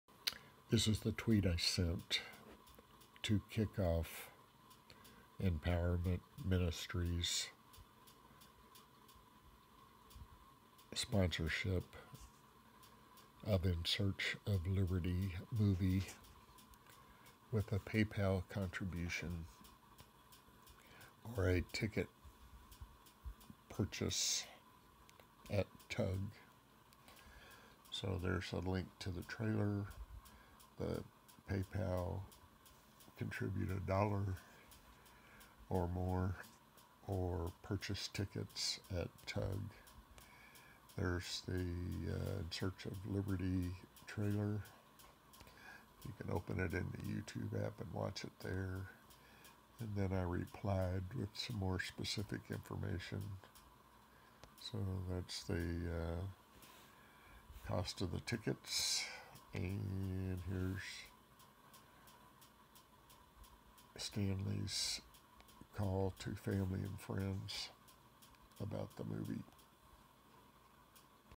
Empowerment Ministries educates to restore Liberty in Texas March 20 2018 8pm 20180219_035244
Stanley Cofer and Empowerment Ministries sponsors a restoration of Liberty in Texas and America